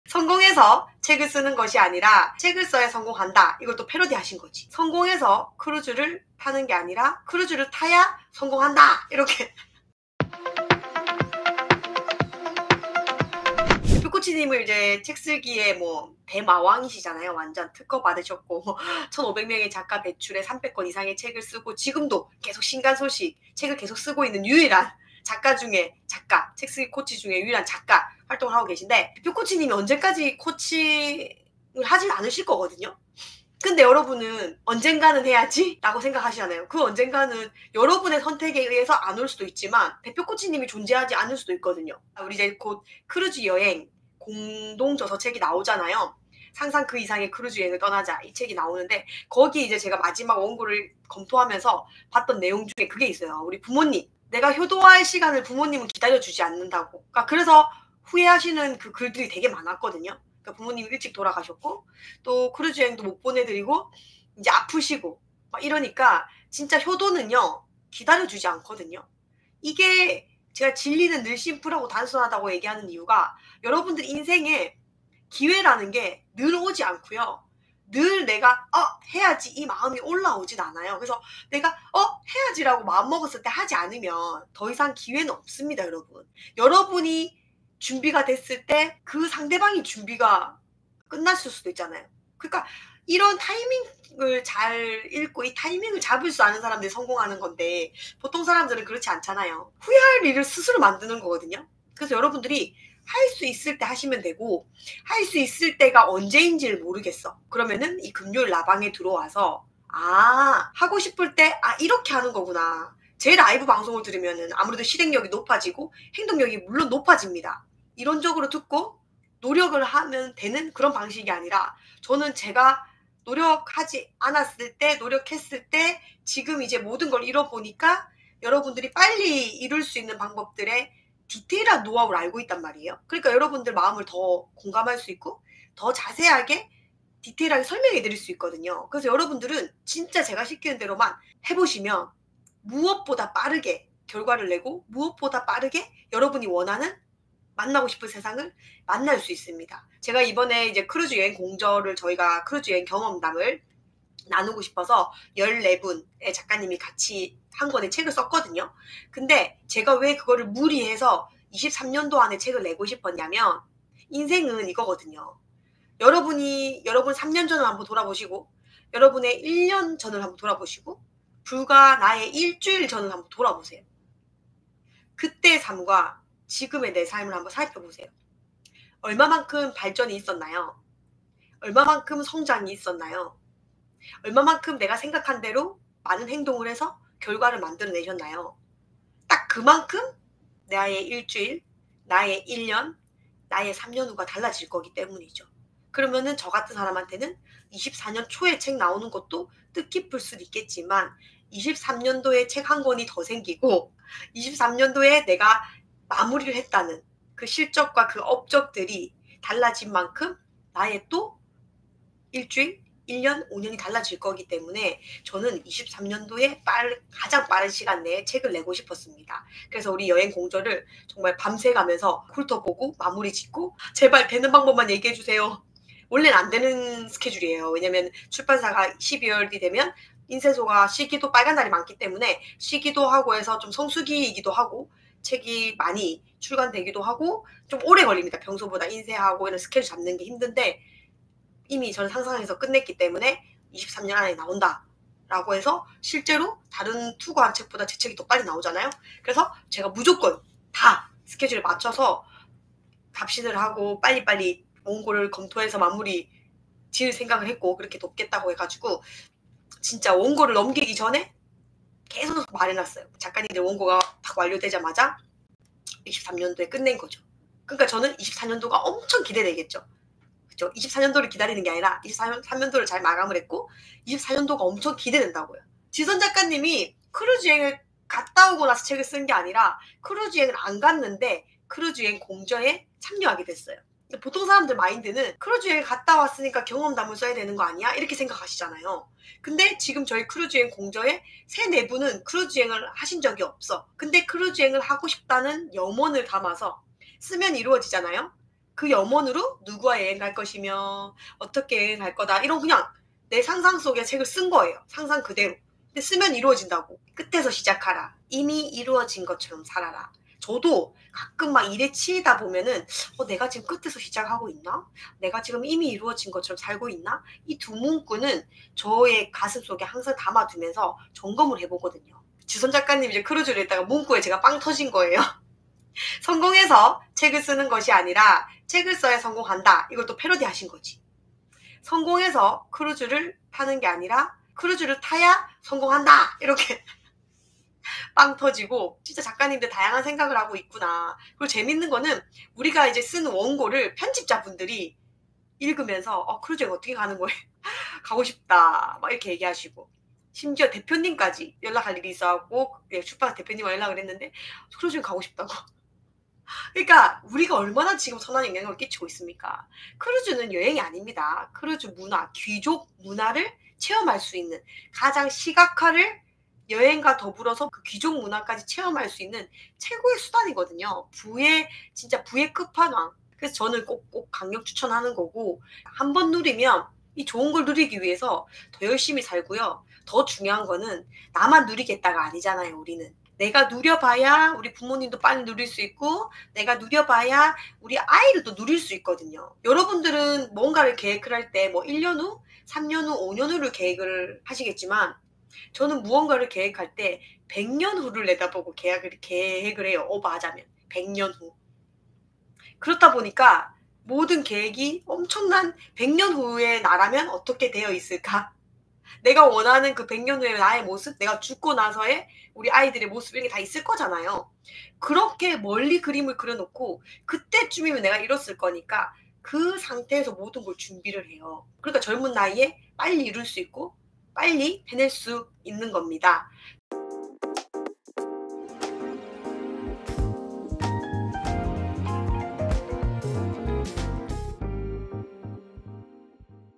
대운의 징조! 무식하게 반복하세요 | 부와 행운을 끌어당기는 우주의 법칙 저자
나, 권대표는, 크루즈 여행가이고, 사랑이 많은 세아이의 엄마이고, 베스트셀러 작가이고, 자산가이고 만나는 모든 사람에게 영감을 주는 동기부여가 인생라떼 권대표가 궁금하다면?

100만원크루즈 멤버쉽

00:00 인트로
00:14 인생의 기회는 늘 오지 않는다
02:32 지금의 나는 얼마만큼 발전이 있었는가
04:36 이미 이루어진 것처럼 살아라
06:17 100년 후를 내다보고 살아라